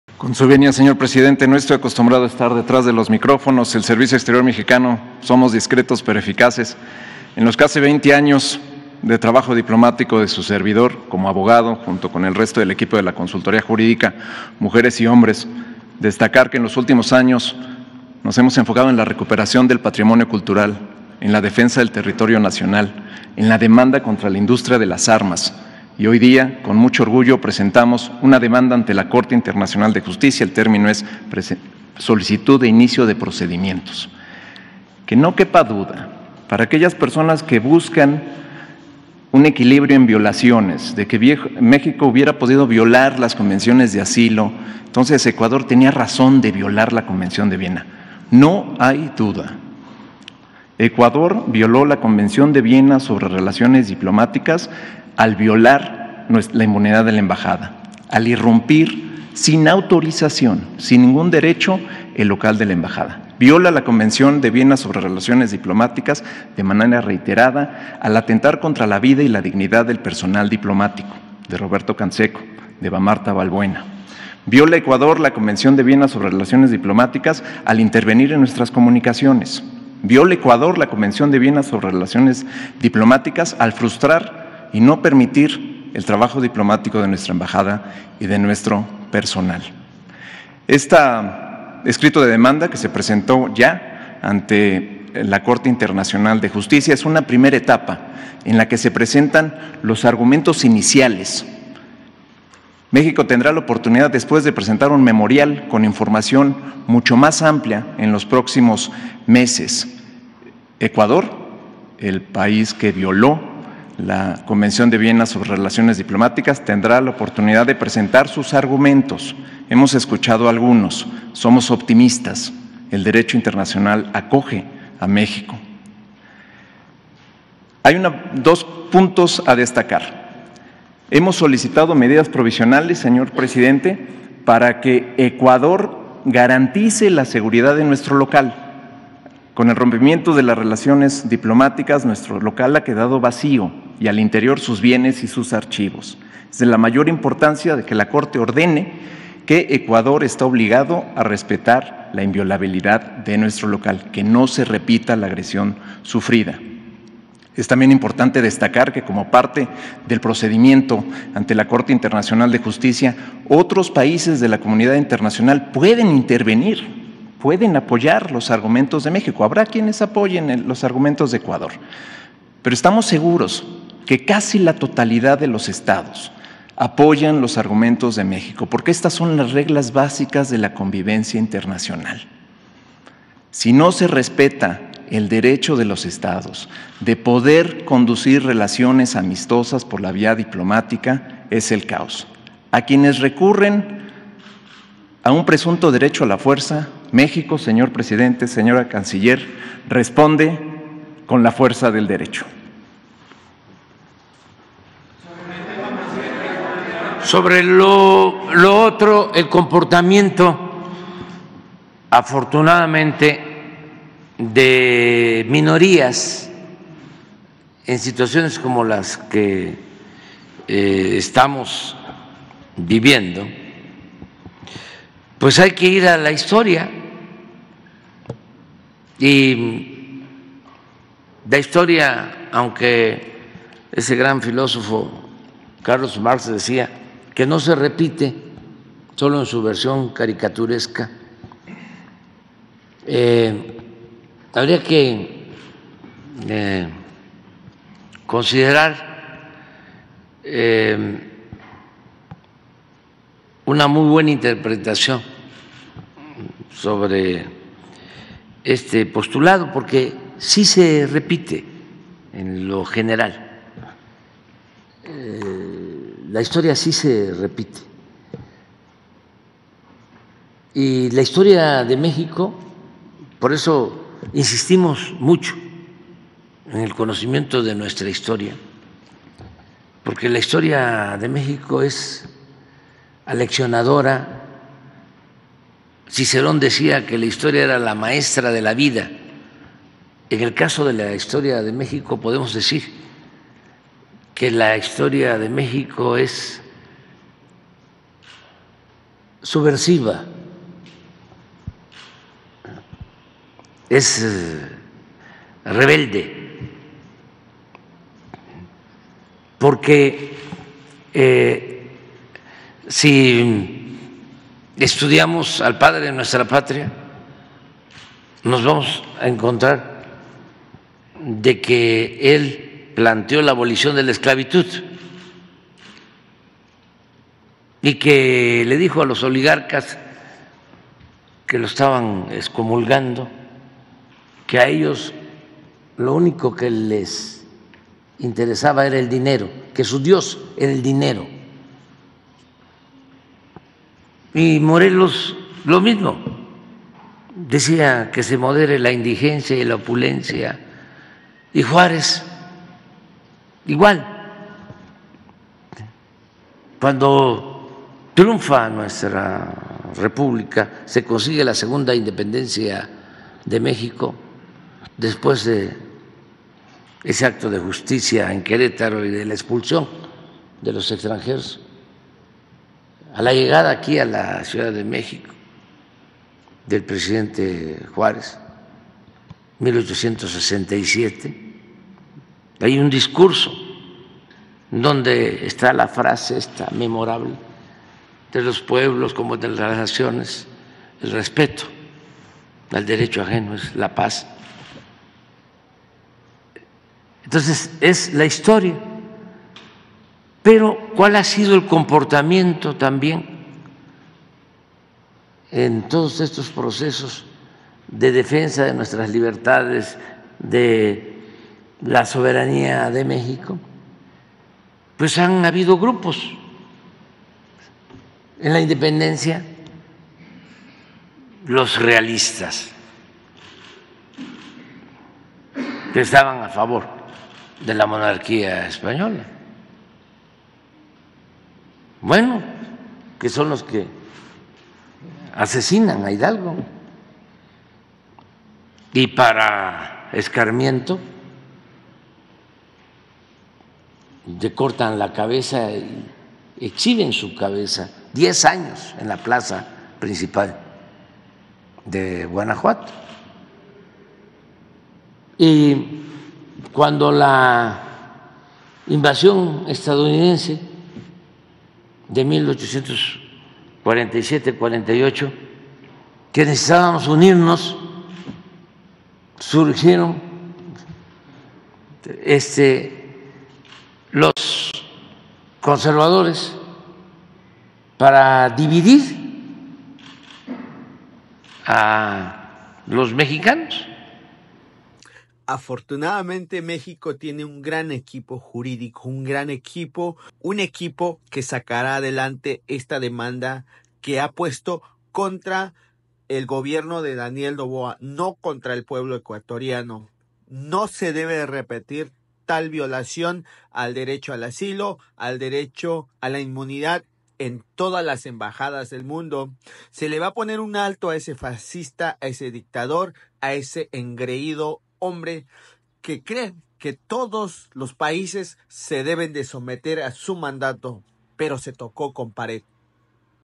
México presentó demanda contra el gobierno de Ecuador, Daniel Noboa
El conflicto entre Ecuador y México ha dividido al mundo.

Por un lado, respaldando a México están EUA, la OEA, ONU, OTAN, BRICS, todos los países de latinoamérica, europa y asia.

Por el otro lado está el PAN, PRI, PRD y LillyTellez